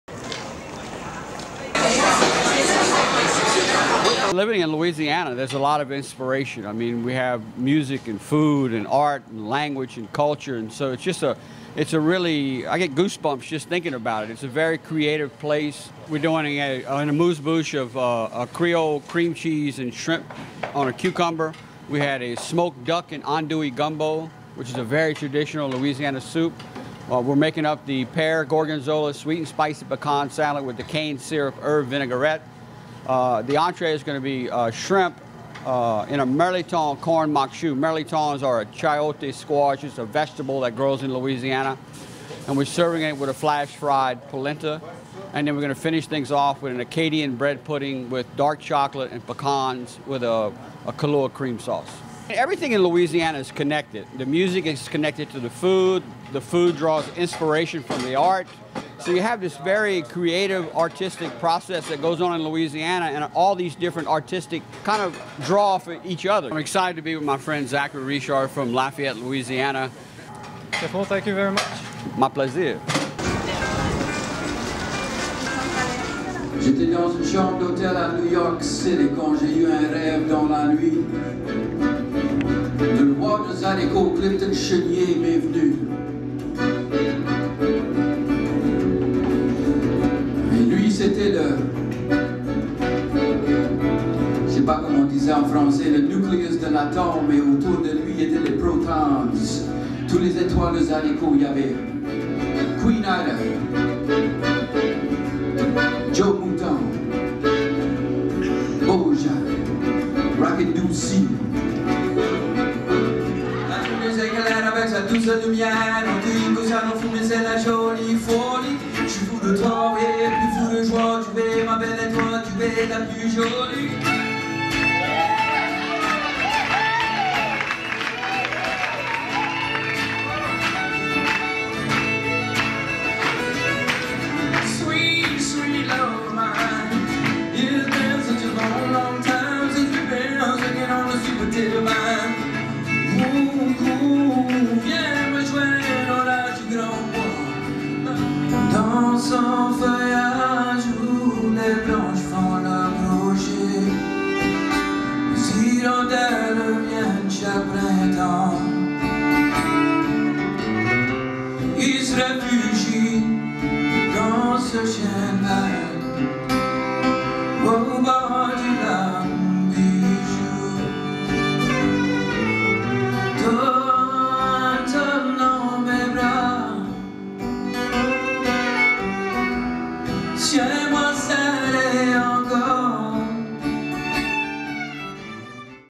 The Great Louisiana Dinner-Show with Zachary Richard and chef Patrick Mould — MONTRÉAL EN LUMIÈRE
A look back at the Great Louisiana Dinner-Show featuring Zachary Richard's concert at L'Astral and chef Patrick Mould's 5-course Cajun menu at Bistro Le Balmoral.